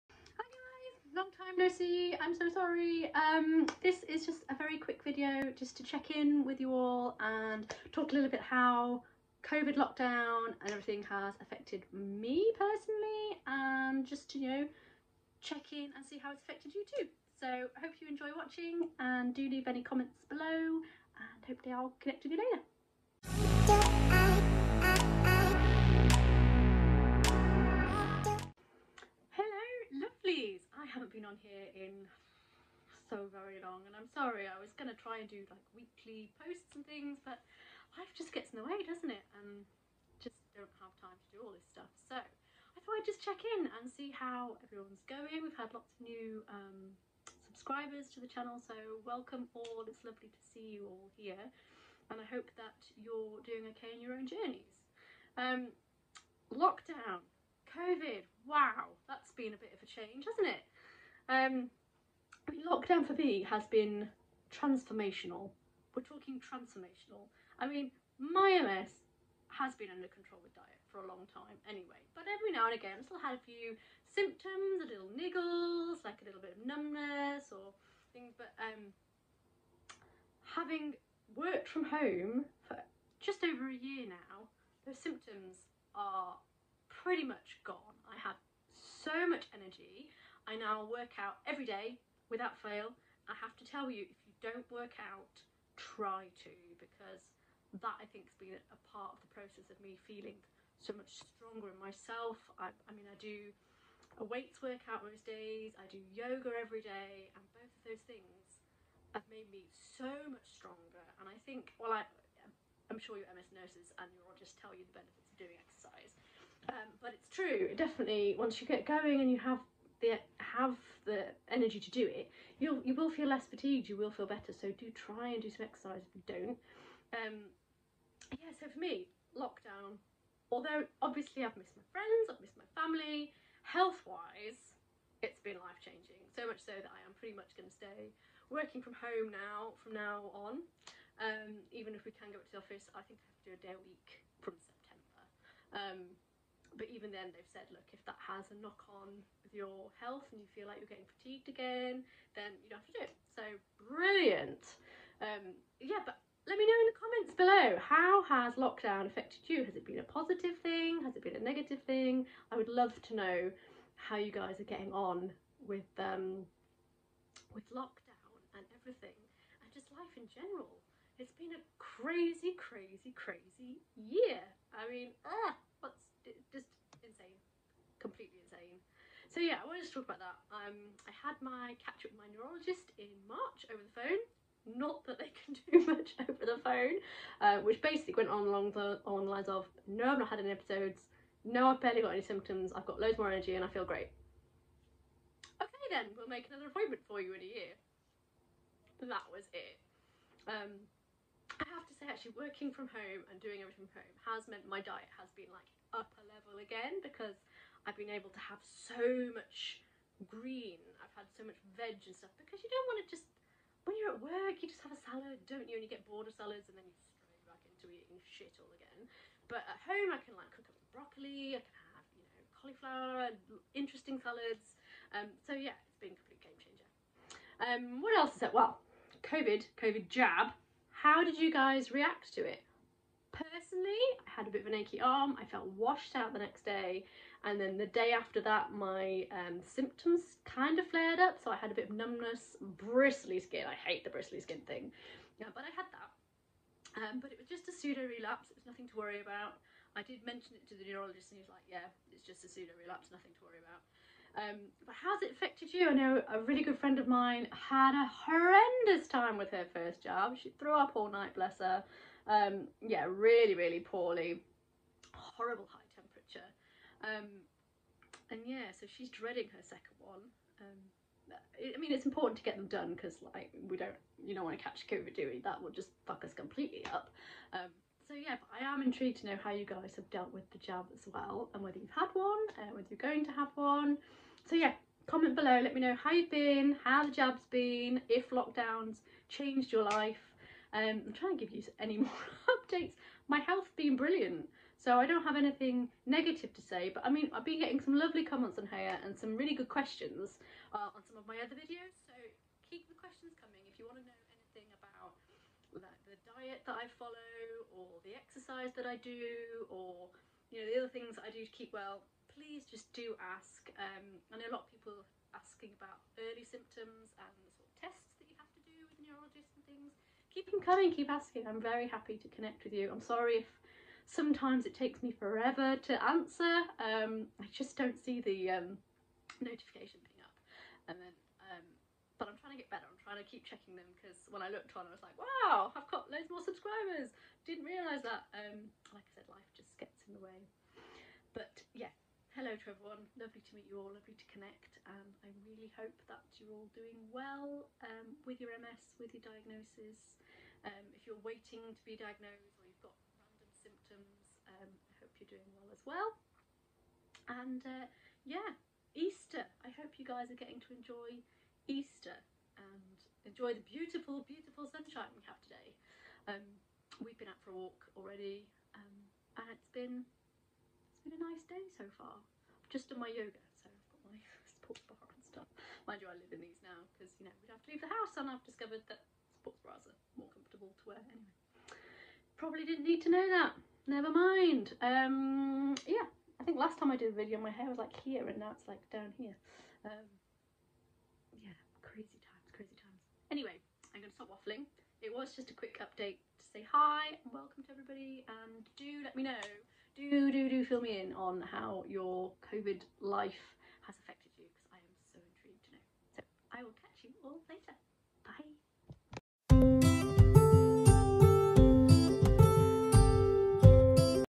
How COVID has been life changing for my MS
In today’s quick video I talk about how Covid lockdown has effected my health and a little bit about how I reacted to the vaccine